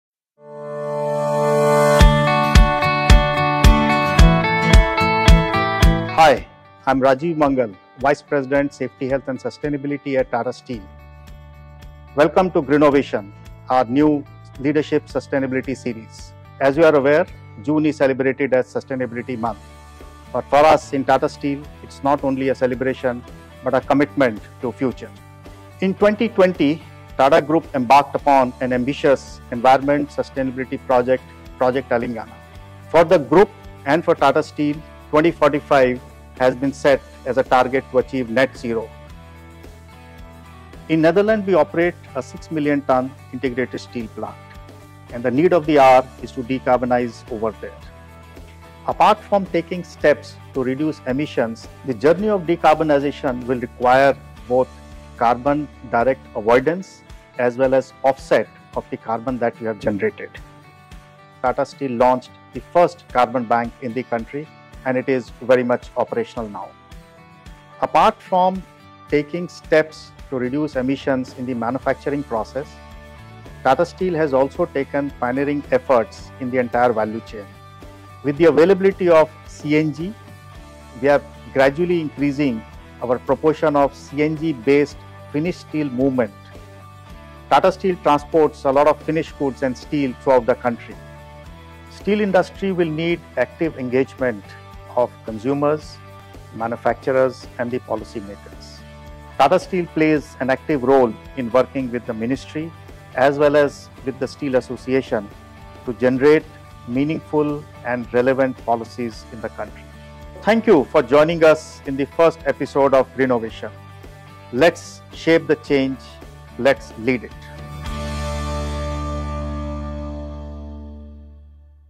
Tata Steel | Greenovation Ep 1: Where Steel Meets Sustainability | The Net Zero Blueprint
Sustainable steel isn’t just an aspiration, it’s the bedrock of our future.

Through our sustainability leadership series - Greenovation, know how we are fulfilling our commitment towards building a greener future.

In the first episode, Rajiv Mangal, Vice President – Safety, Health & Sustainability, Tata Steel,  outlines the foundation of our net zero journey.